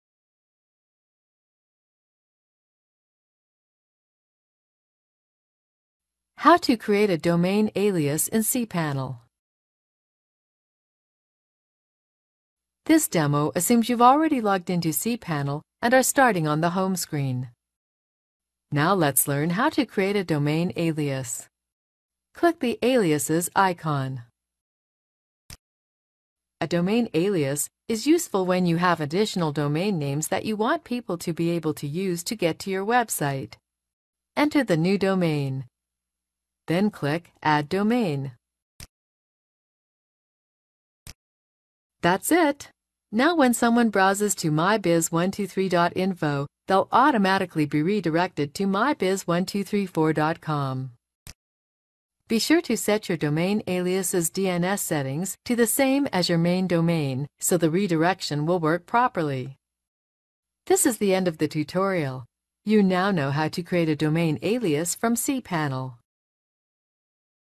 How to create a domain alias in cPanel - HOSTIMUL
This demo assumes you've already logged in to cPanel, and are starting on the home screen.

Now let's learn how to create a domain alias.

Click the Aliases icon.

A domain alias is useful when you have additional domain names that you want people to be able to use to get to your website.

Enter the new domain, then click Add Domain.

That's it! Now when someone browses to mybiz123.info, they'll automatically be redirected to mybiz1234.com.

Be sure to set your domain alias's DNS settings to the same as your main domain, so the redirection will work properly.

This is the end of the tutorial. You now know how to create a domain alias from cPanel.